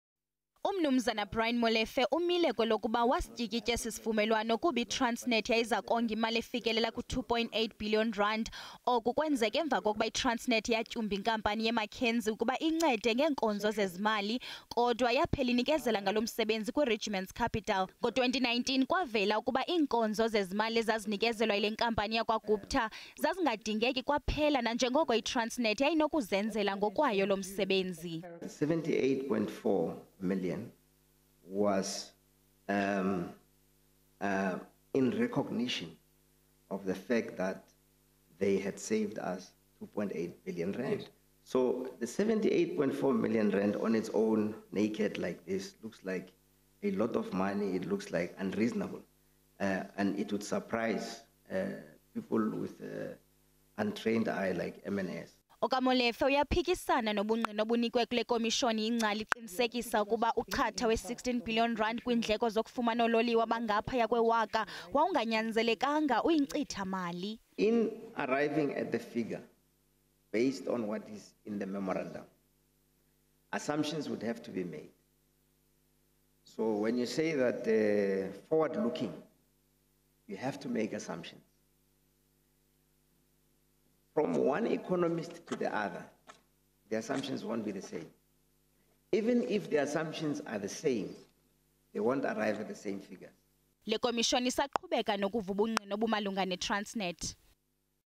UMolefe unike ubungqina ngeTransnet
Obesakuba ngumlawuli oyintloko kwaTransnet umnumzana Brian Molefe uziphikile izityholo zokuba isiqingatha seSeventy Eight Million Rand eyaafunyanwa yinkampani yakwaGupta iRegiments Capital yaya kumnumzana Salim Essa. Izolo okaMolefe uphikile ukuba uyamazi umnumzana Essa nowayesebenzisana nabakwaGupta kwezoshishino.

Ukufumana ezinye iindaba, ndwendwela i-sabcnews.com, SABCNews SABCIindaba StateCaptureInquiry kumajelo onxibelelwano.